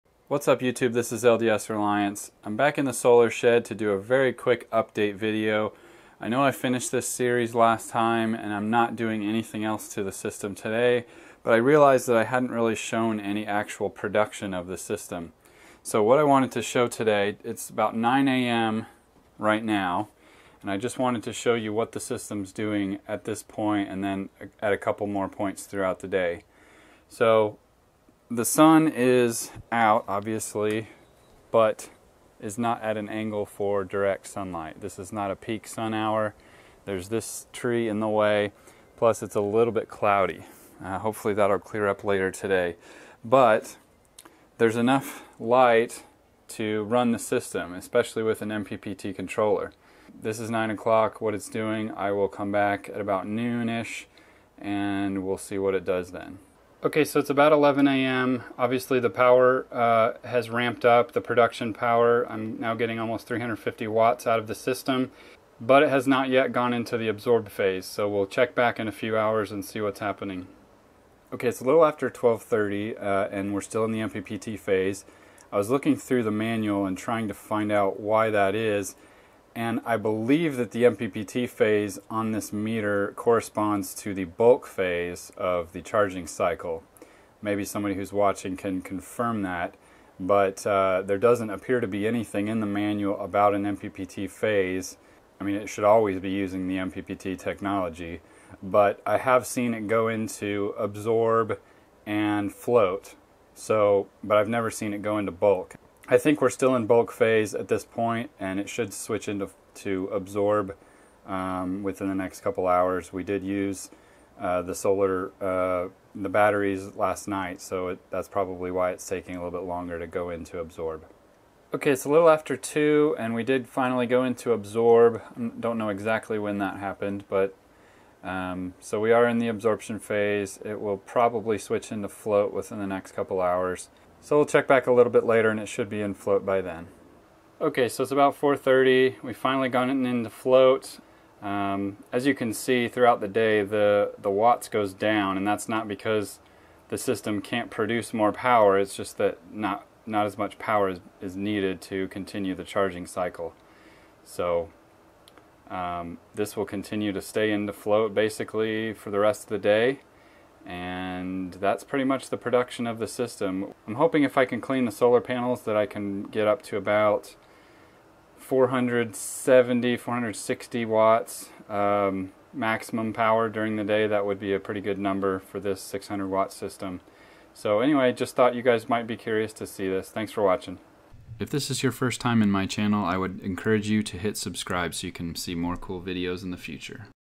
New (Redone) Solar Shed Energy Production
OK, so I redid the solar shed, but how much energy is it producing?  Lets find out.

In this short video, I check in on the redone solar shed system at various points in the day to check on the production numbers and watch the system move through the various phases of the battery charging cycle.